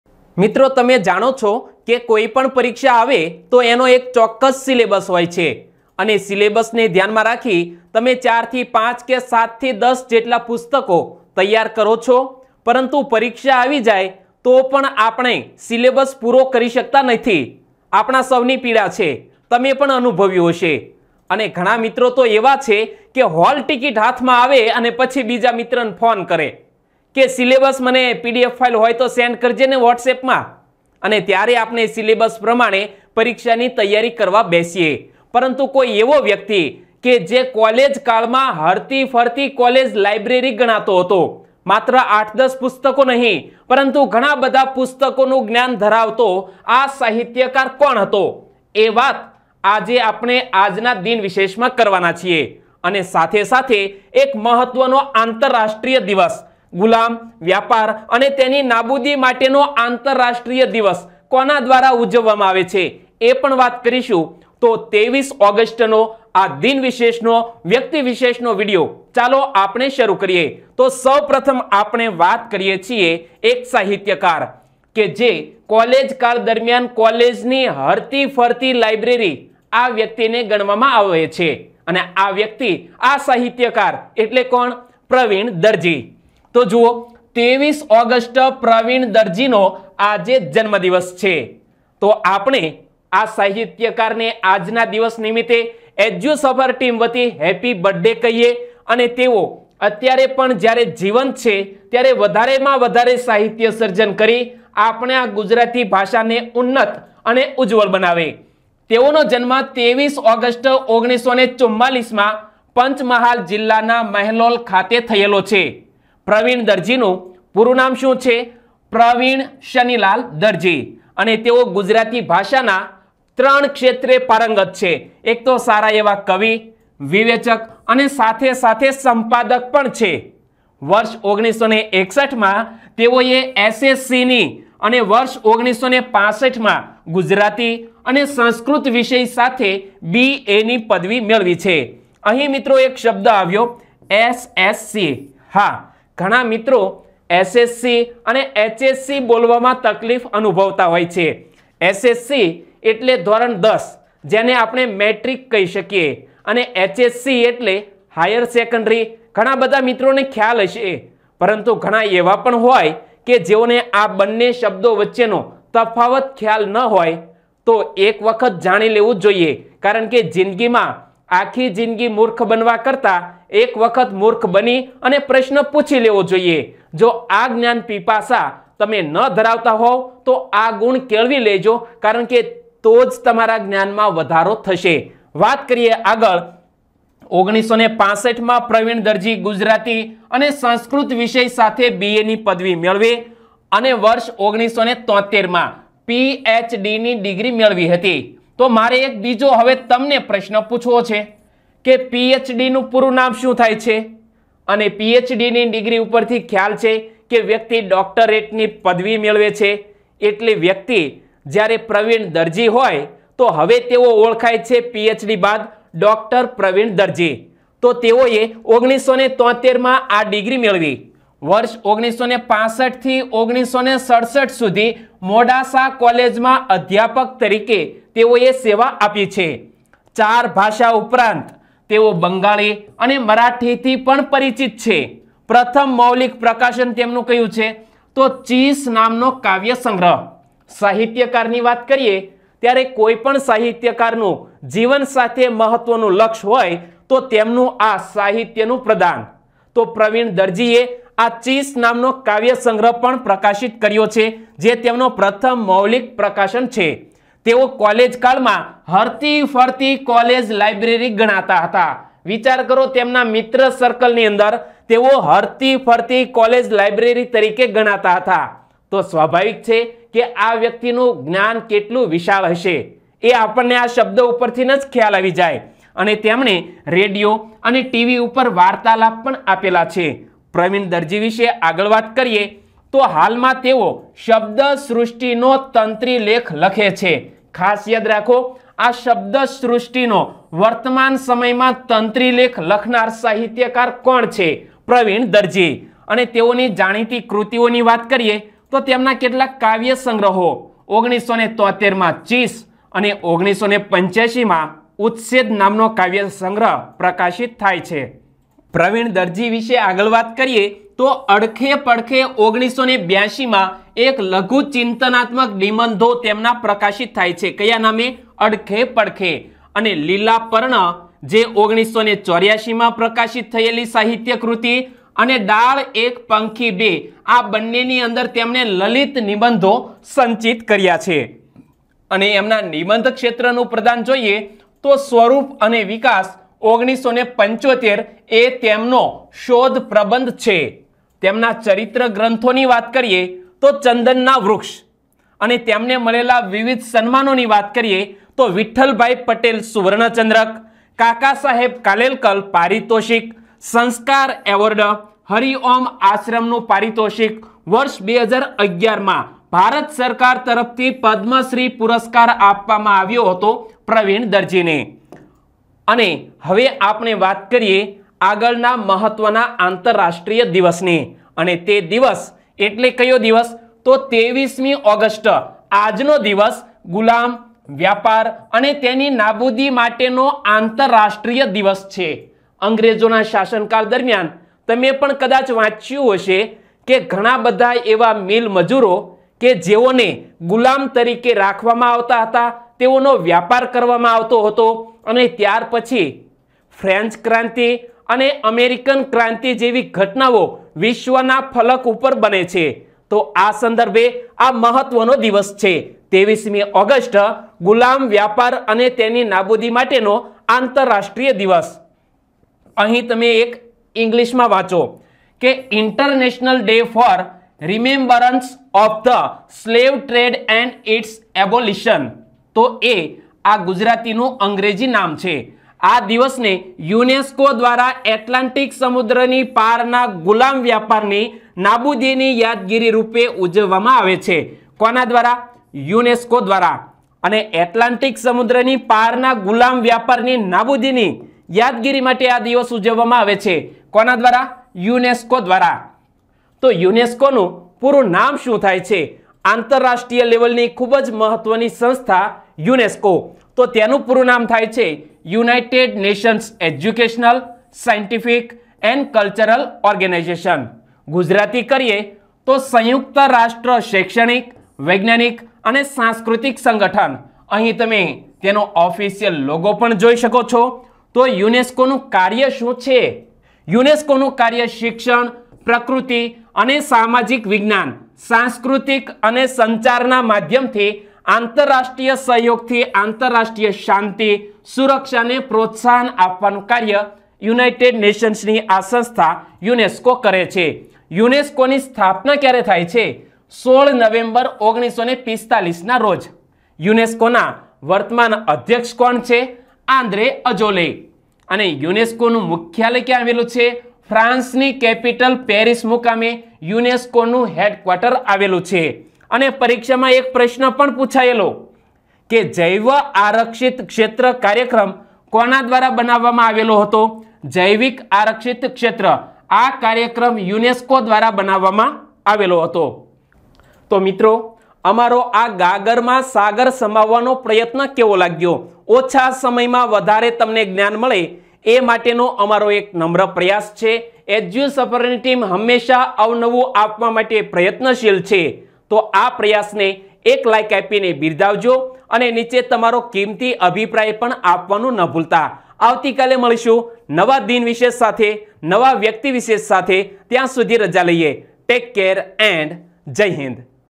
23 August| Pravin Darji | International Day for the Remembrance of the Slave Trade and its Abolition
પ્રસ્તાવિક ચર્ચા
દિન વિશેષ
23 ઓગસ્ટ, "પ્રવીણ દરજીનો" જન્મદિવસ.
23 ઓગસ્ટ,"ગુલામ,વ્યાપાર,અને તેની નાબૂદી માટેનો" આંતરરાષ્ટ્રીય દિવસ.